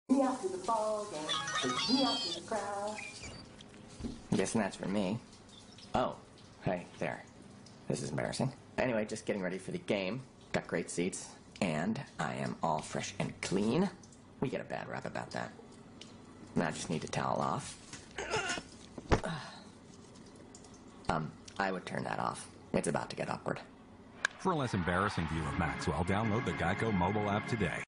GEICO Mobile App TV Commercial Featuring Maxwell the Piggy iSpot tv
One of the Funniest GEICO Commercial hope you like the video.